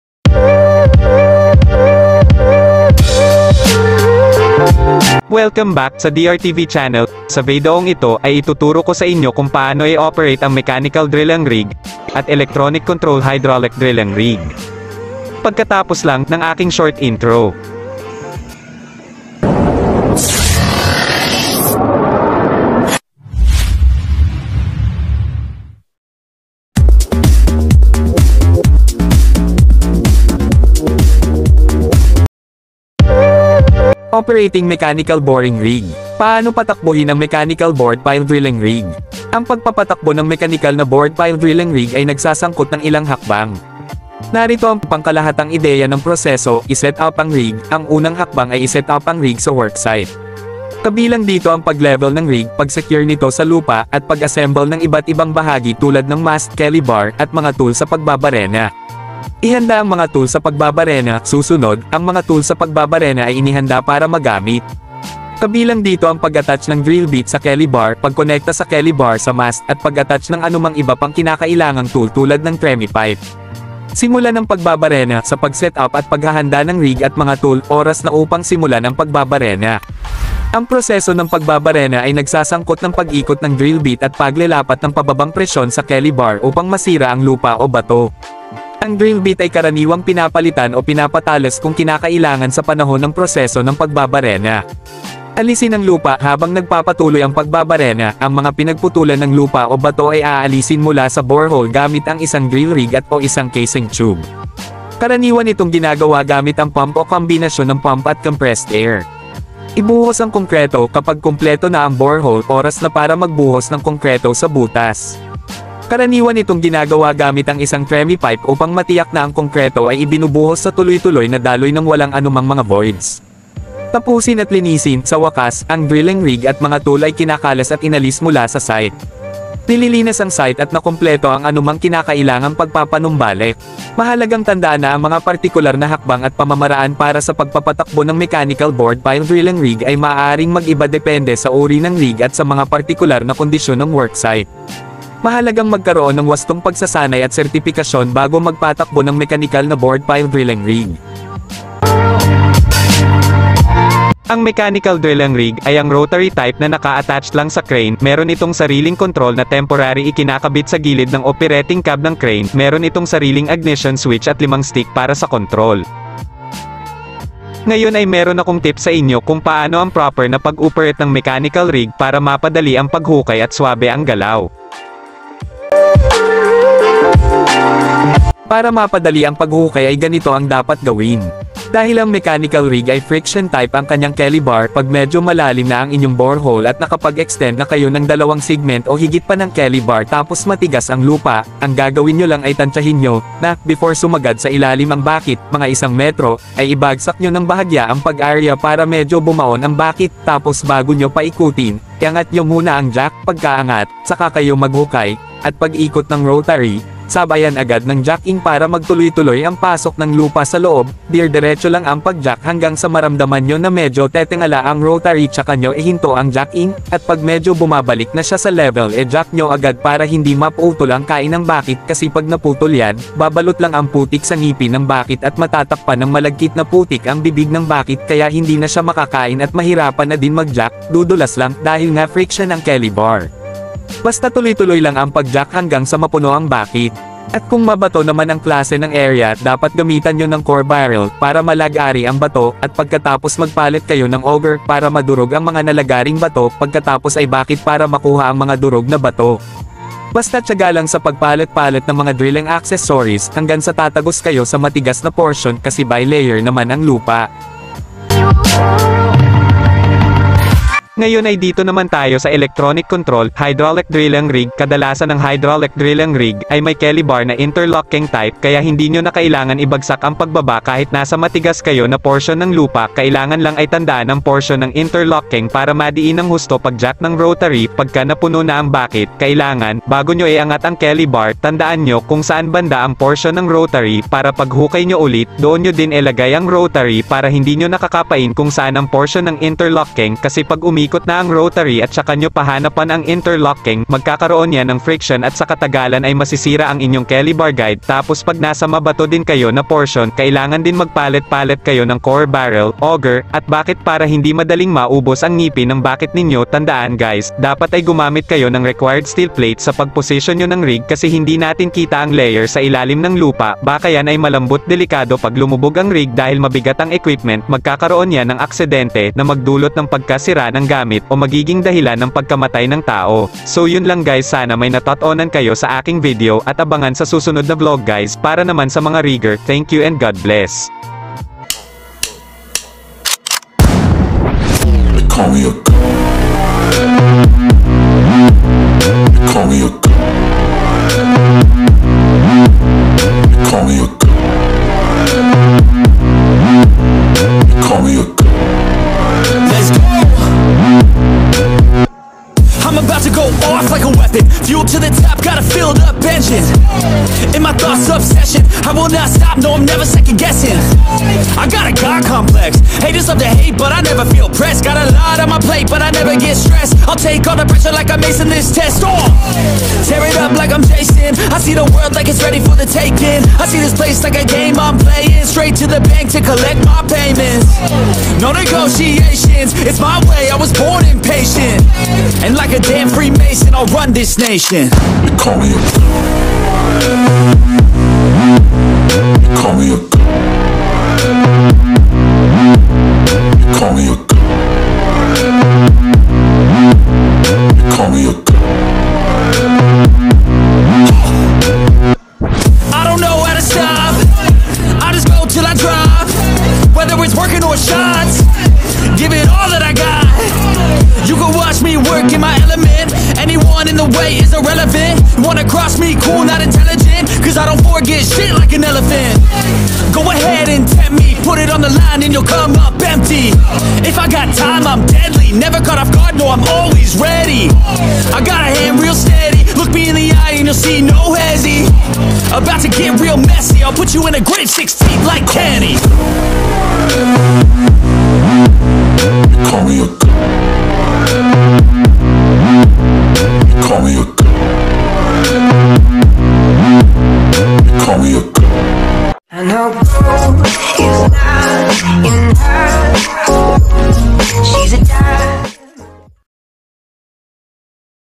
PAANO E OPERATE ANG BORED PILE RIG (Rig tutorial)@DRTV CHANNEL
#@DRTV CHANNEL has content. Movie, vlogs, boxing sport, and crane operators tips ang tutorials. This is related on how to proper operating of  equipments.Operating a bored pile rig can be a complex task that requires specialized training and experience. However, here are some general steps to operate a bored pile rig:

Pre-Operational Checks: Before starting the rig, it is important to conduct a pre-operational check to ensure that all components of the rig are functioning properly. This includes checking the hydraulic system, engine oil level, fuel level, and all safety devices.